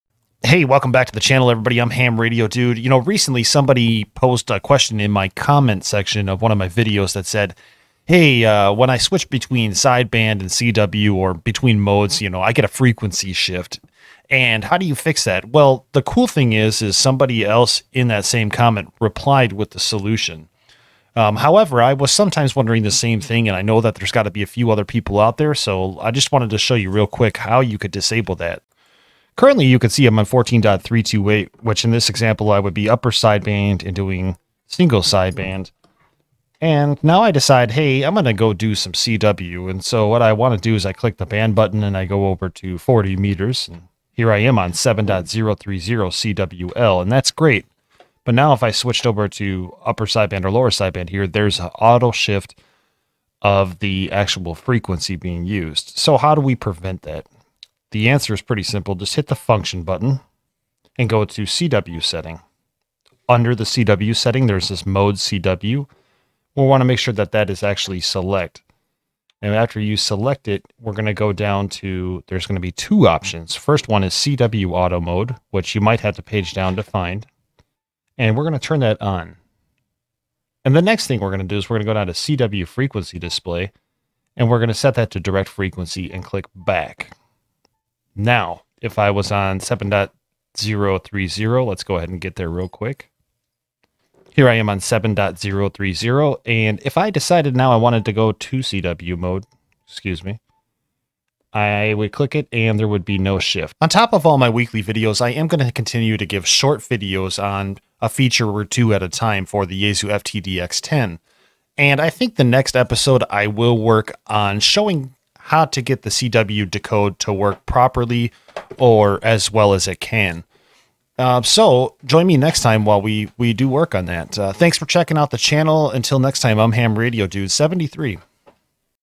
Yaesu FT-DX10 - Enabling CW Auto Mode / CW Freq Display
How to enable CW Auto Mode and Enable Direct Frequency Display on the Yaesu FT-DX10